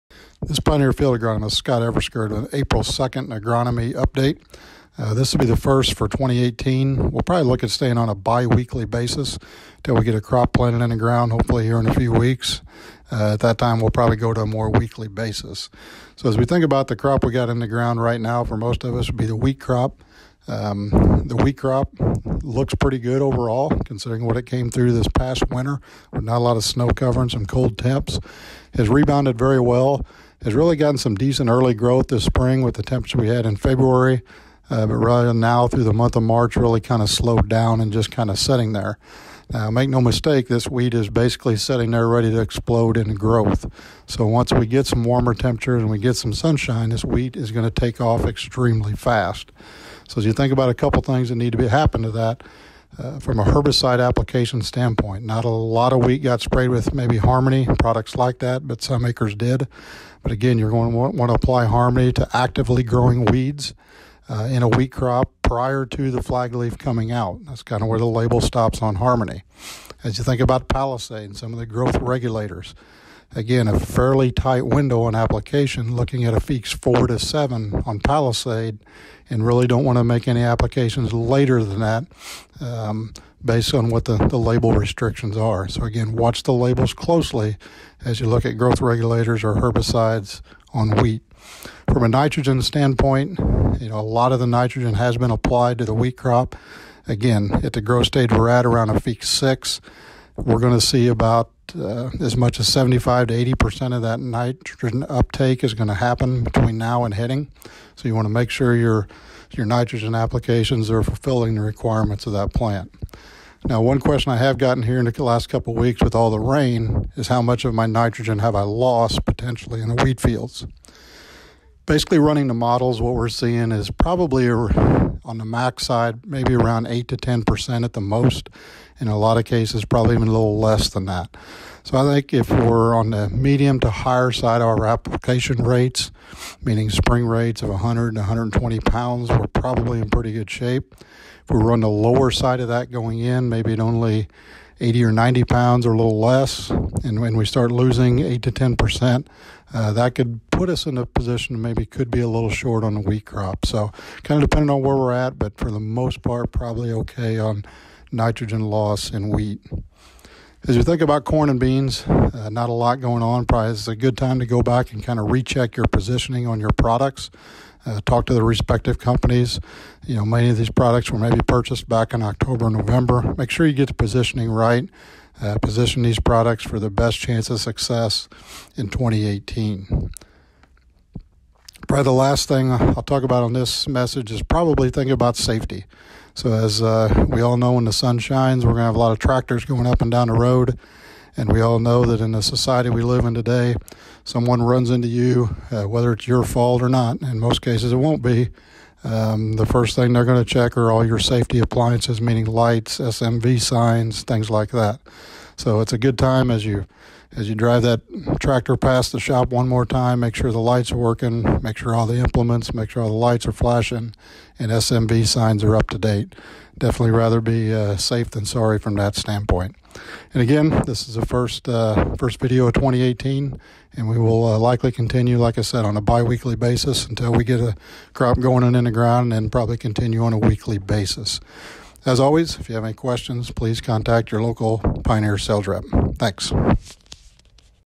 U'R Fields 4 2 2018
Agronomist Scott Eversgerd describes the wheat crop conditions in Southern IL. Wheat growth, herbicide application on wheat, positioning products and safety.